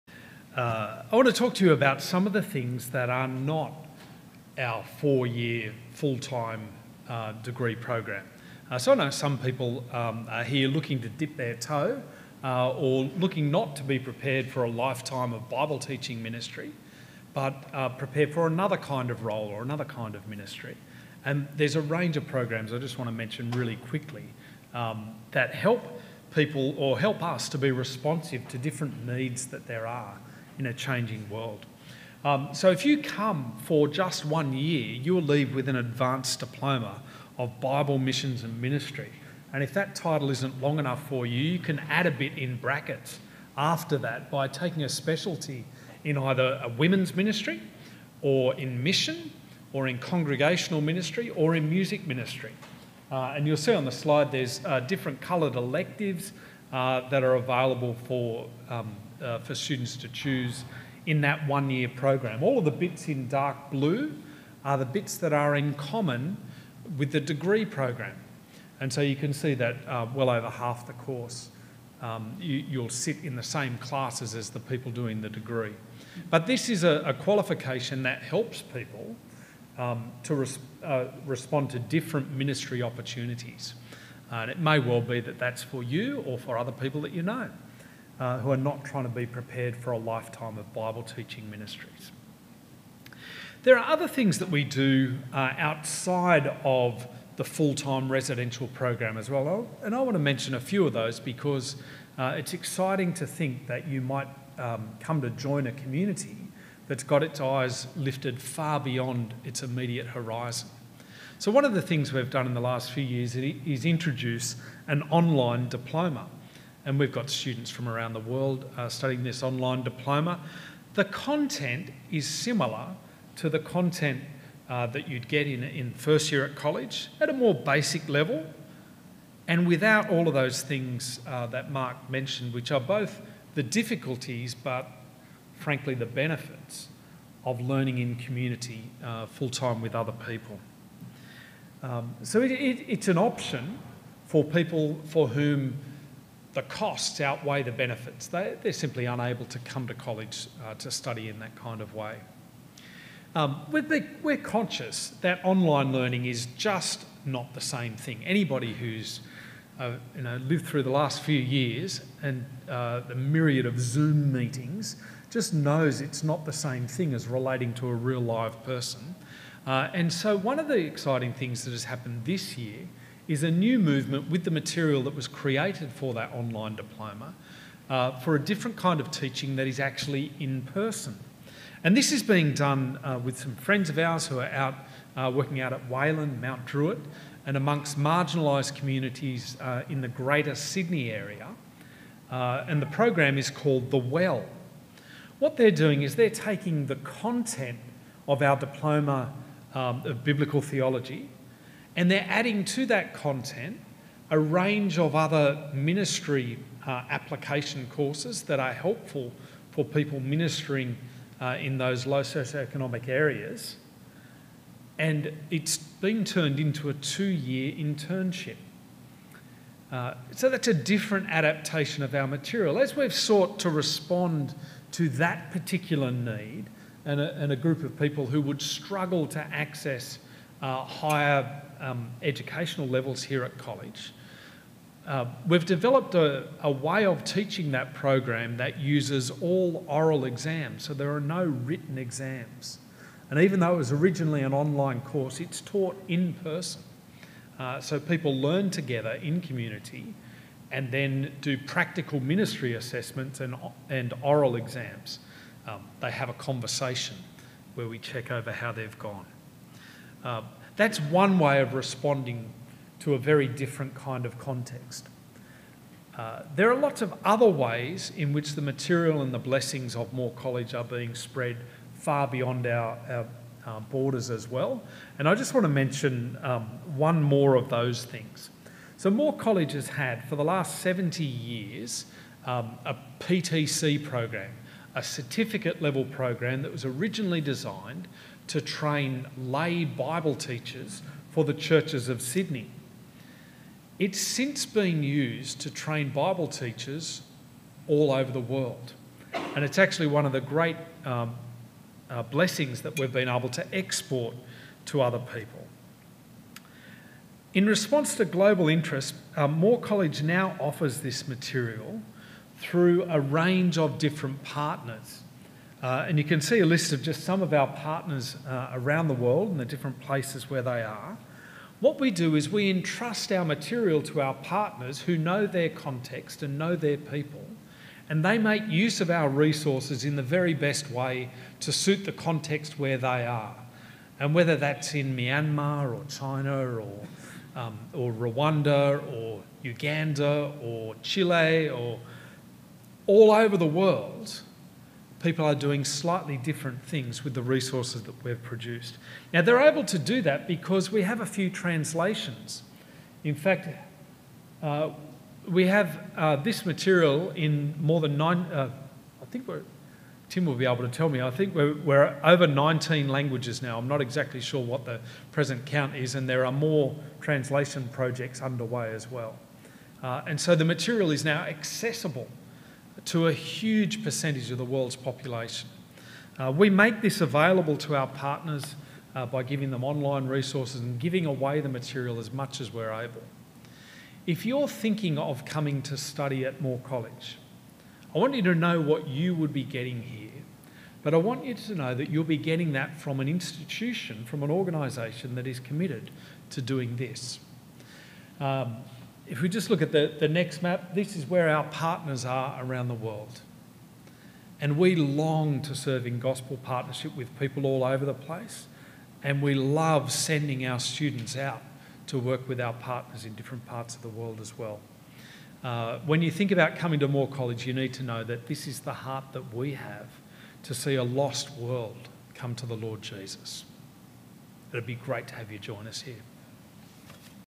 The diversity of Moore College modes of study - Simon Gillham
In this clip from the recent Open Night held at Moore College in May, Simon Gillham discusses the various programs and modes of study offered by Moore College that are not part of their four-year full-time degree program. These programs are designed to cater to different needs and are responsive to the changing world.

for details on upcoming Open Events at Moore College.